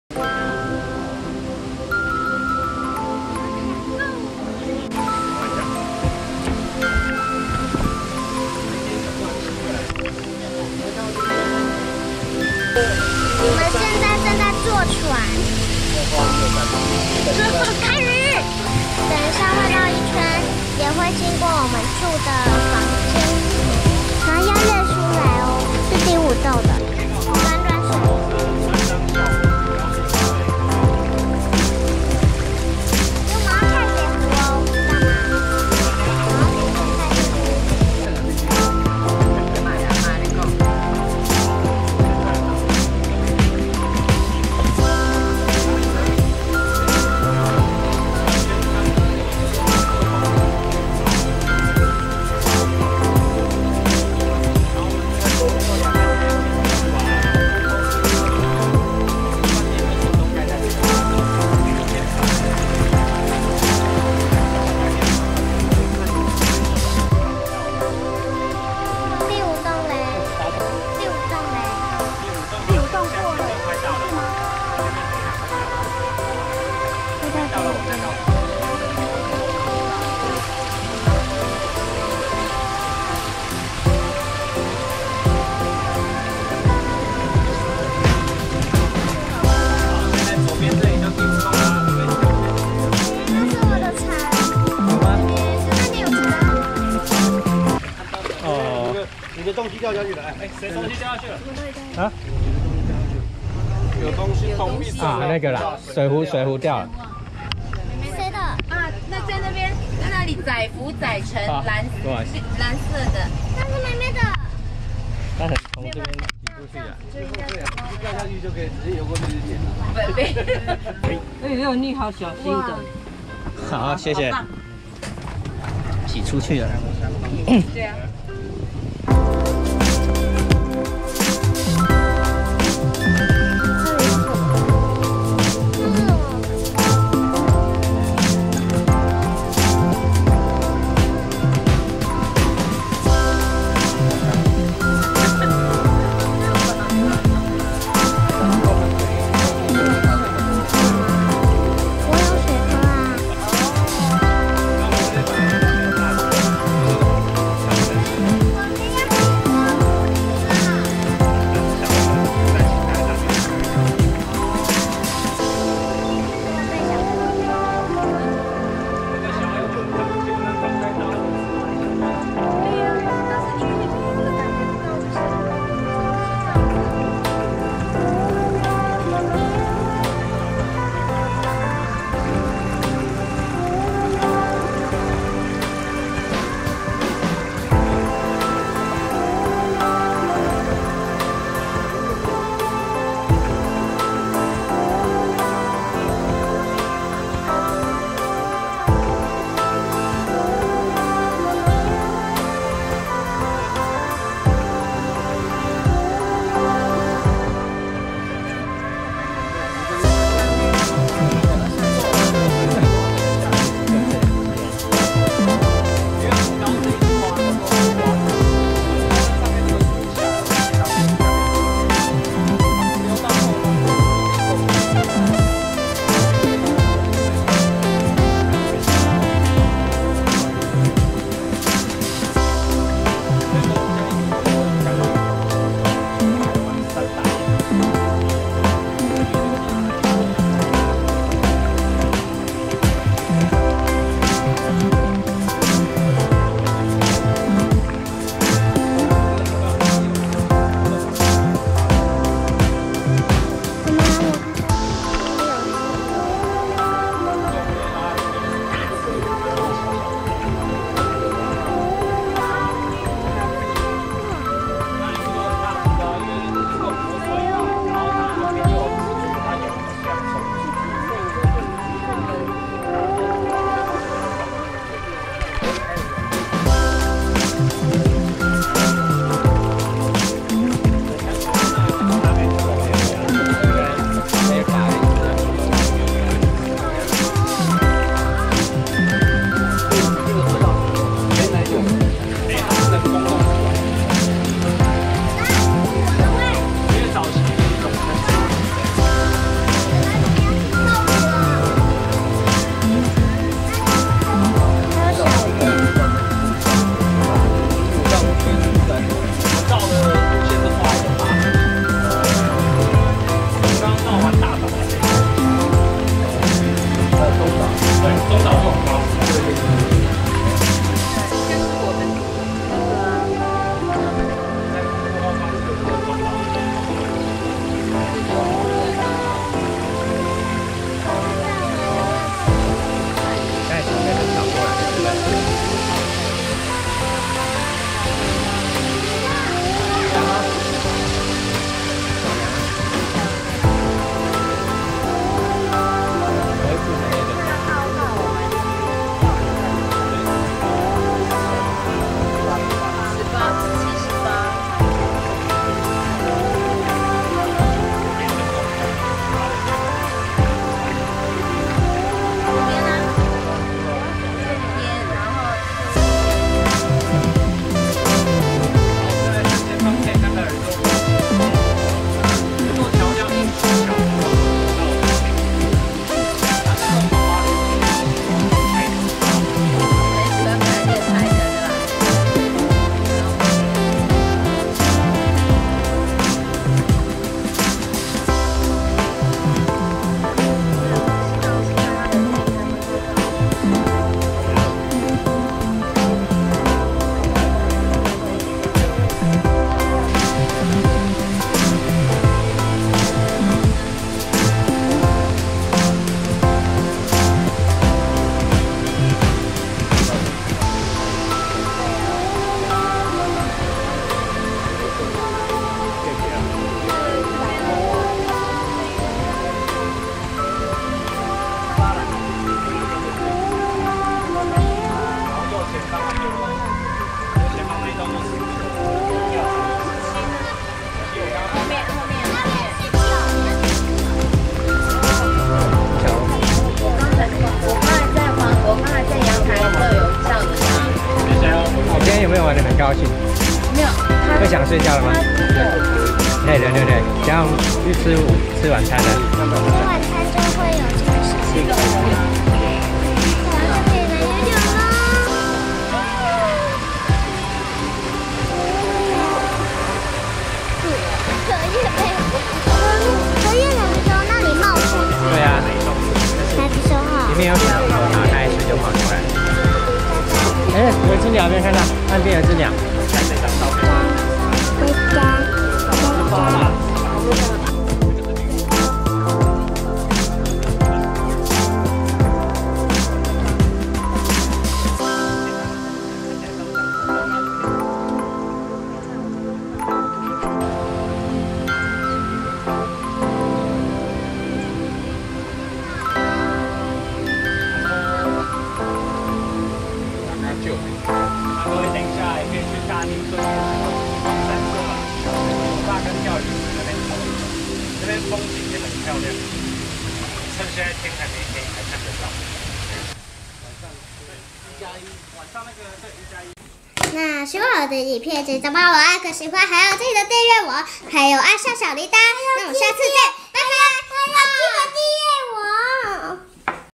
[端午連假]_花蓮_EP15_理想大地_第二次座遊艇遊應許之河_190608
又來坐一次遊艇遊應許之河